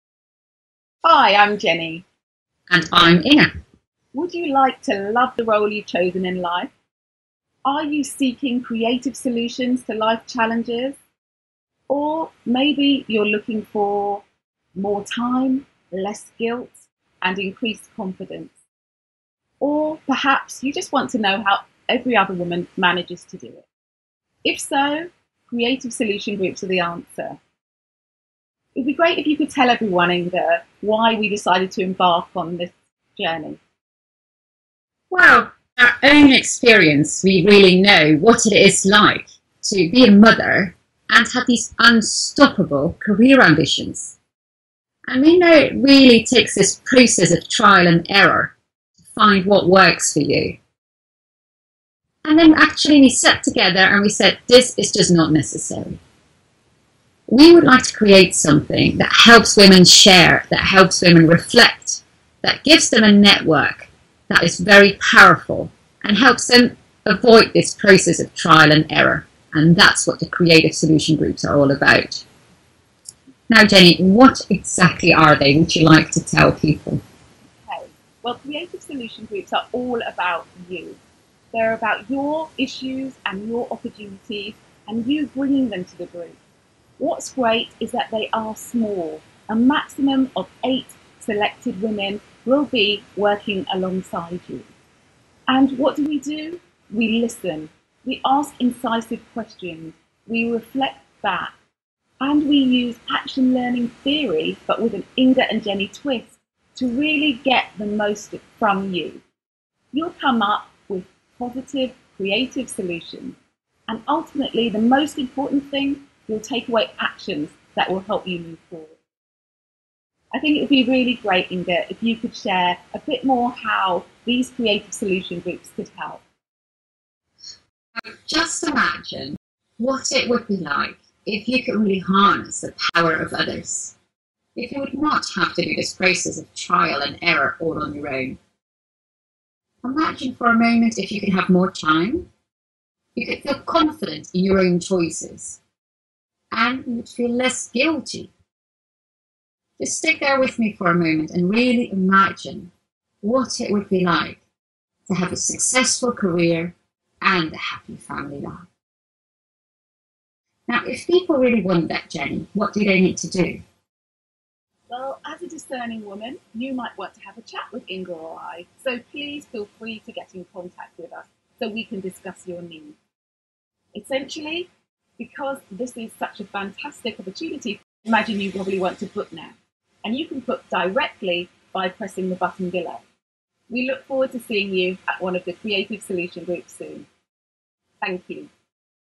Creative Solution Groups
Find out more about this innovative development especially for working women and working Mums. The brainchild of Inge Woudstra of Mum & Career and Jenny Garrett, author of Rocking Your Role